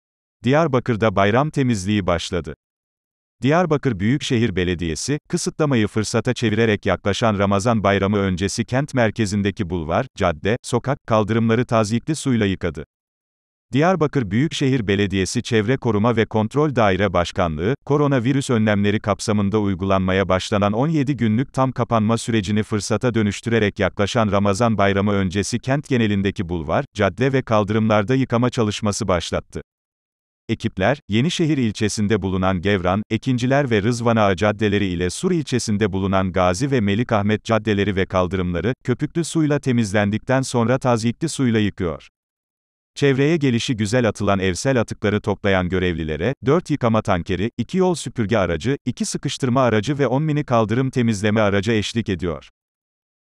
Diyarbakır’da bayram temizliği başladı
Diyarbakır Büyükşehir Belediyesi, kısıtlamayı fırsata çevirerek yaklaşan Ramazan Bayramı öncesi kent merkezindeki bulvar, cadde, sokak, kaldırımları tazyikli suyla yıkadı.
Diyarbakır Büyükşehir Belediyesi Çevre Koruma ve Kontrol Daire Başkanlığı, korona virüs önlemleri kapsamında uygulanmaya başlanan 17 günlük tam kapanma sürecini fırsata dönüştürerek yaklaşan Ramazan Bayramı öncesi kent genelindeki bulvar, cadde ve kaldırımlarda yıkama çalışması başlattı. Ekipler, Yenişehir ilçesinde bulunan Gevran, Ekinciler ve Rızvanağa caddeleri ile Sur ilçesinde bulunan Gazi ve Melikahmet caddeleri ve kaldırımları, köpüklü suyla temizlendikten sonra tazyikli suyla yıkıyor.
Çevreye gelişi güzel atılan evsel atıkları toplayan görevlilere, 4 yıkama tankeri, 2 yol süpürge aracı, 2 sıkıştırma aracı ve 10 mini kaldırım temizleme aracı eşlik ediyor.